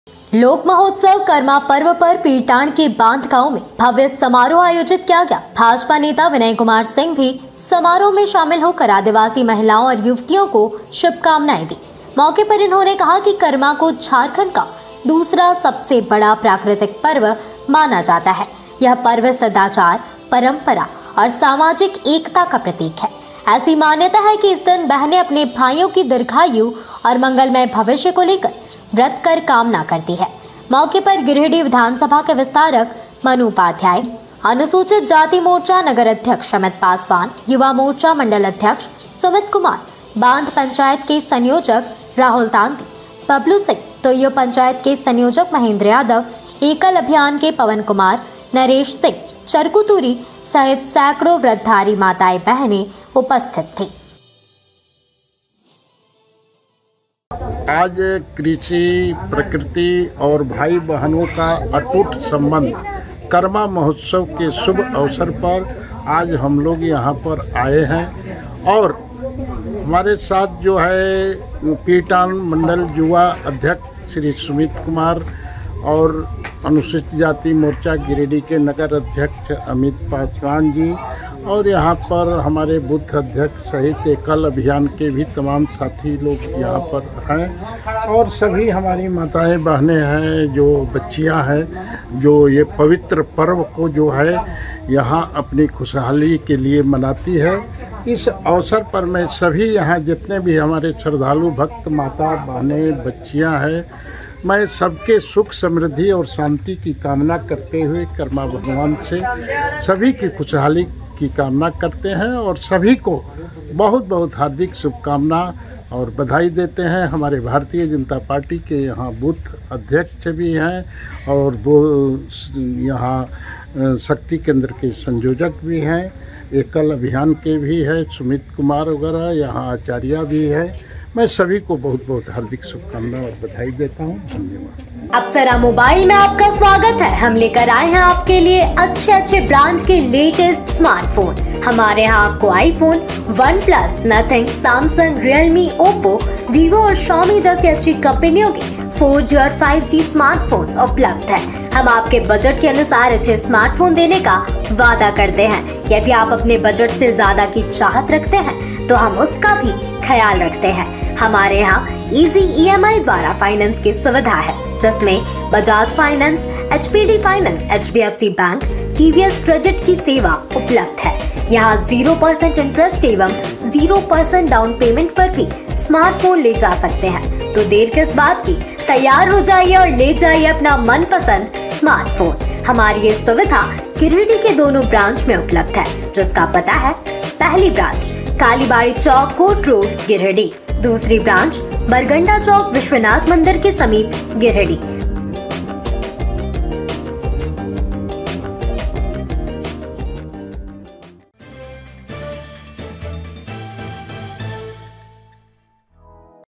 पीरटांड़ के बांध ग्राम में करमा पूजा में भव्य समारोह का हुआ आयोजन
पीरटांड़ के बांध ग्राम में करमा पूजा में भव्य समारोह का हुआ आयोजन, भाजपा नेता विनय कुमार सिंह भी हुए शामिल